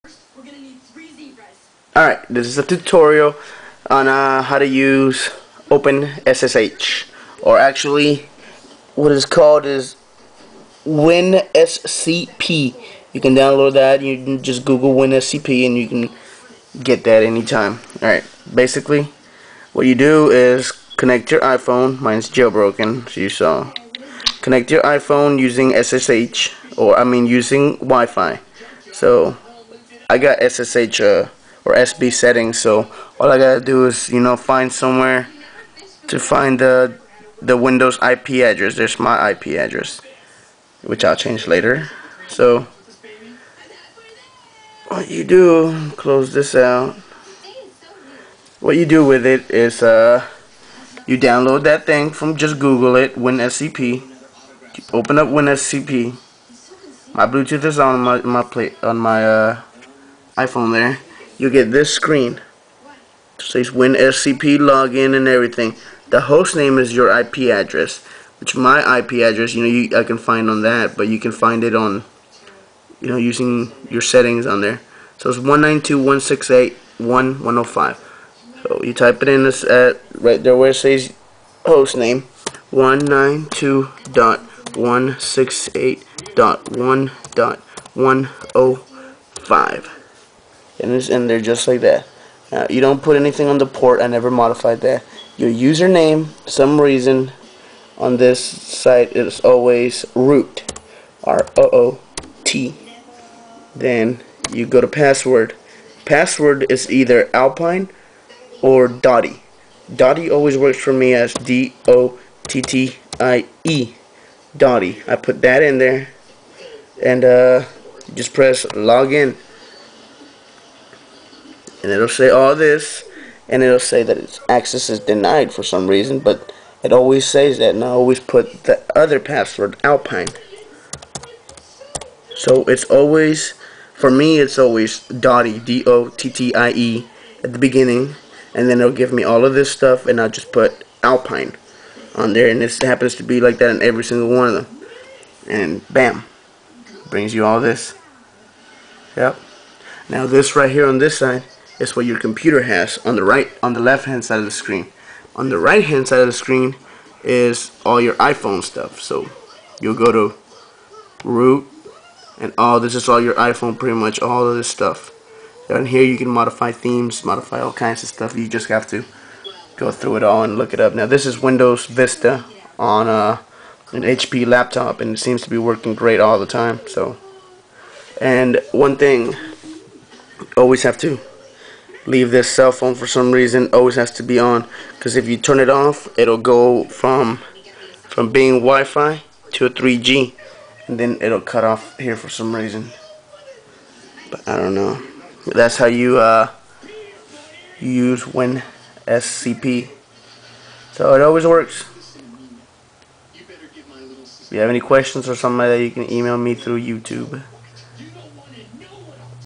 how to use winSCP with iphone 3g
this is a small tut. on how to connect a iphone 3g using winSCP. i used a windows vista laptop for this